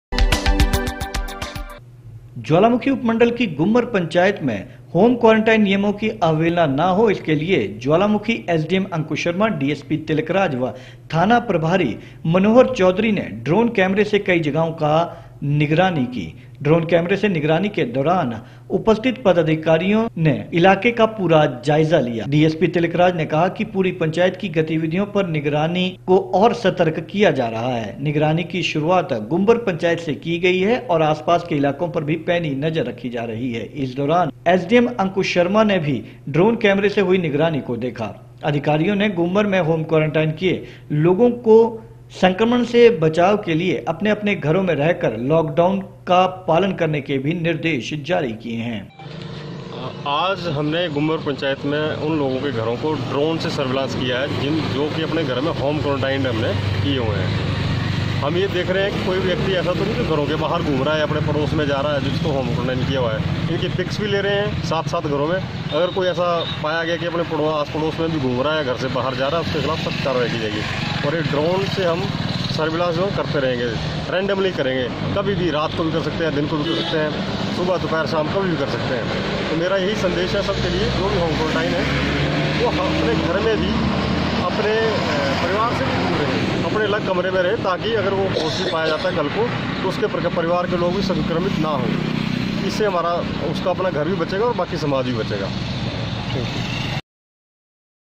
14 may 05 क्वारन्टीन नियमों की पालना के लिए गुम्मर में ड्रोन की नजरप्रसाशन की पूरी टीम रही मौजूद
ज्वालामुखी उपमण्डल की गुम्मर पंचायत में होम क्वारन्टीन नियमों की अवहेलना न हो इसके लिए आज ज्वालामुखी एसडीएम अंकुश शर्मा, डीएसपी तिलकराज, थाना प्रभारी मनोहर चौधरी आदि ने ड्रोन कैमरे से कई जगहों की निगरानी की ड्रोन कैमरे से निगरानी के दौरान उपस्थित पदाधिकारीडीएसपी तिलकराज ने कहा पूरी पंचायत की गतिविधियों की निगरानी की गई, क्योंकि सतर्कता बहुत जरूरी है।निगरानी की शुरुआत गुम्मर पंचायत से की गई और आस पास के इलाकों पर भी नजर रखी गई।इस दौरान एसडीएम अंकुश शर्मा  व डीएसपी तिलकराज ने ड्रोन कैमरे से हो रही निगरानी को देखा। अधिकारियों ने गुम्मर में होम क्वारन्टीन किये गए लोगों को संक्रमण से बचाव के लिए अपने अपने घरों में रह कर लाॅकडाउन का पालन करने का निर्देश दिया।